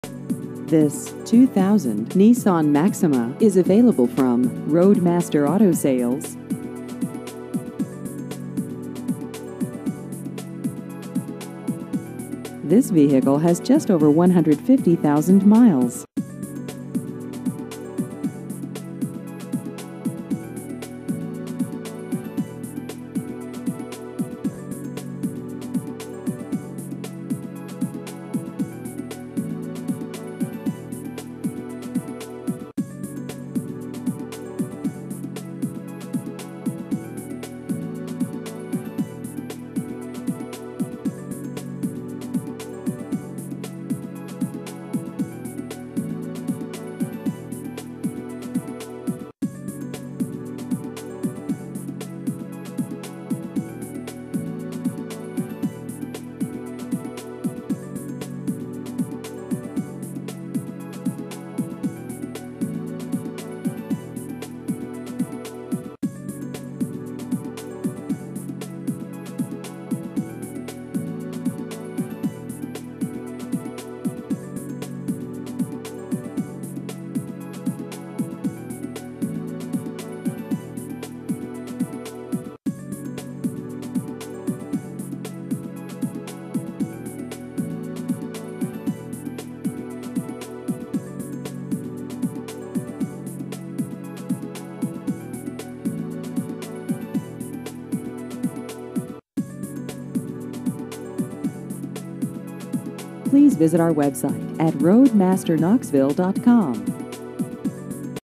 2000 Nissan Maxima Used Cars Knoxville TN
This 2000 Nissan Maxima is available from Roadmaster Auto Sales.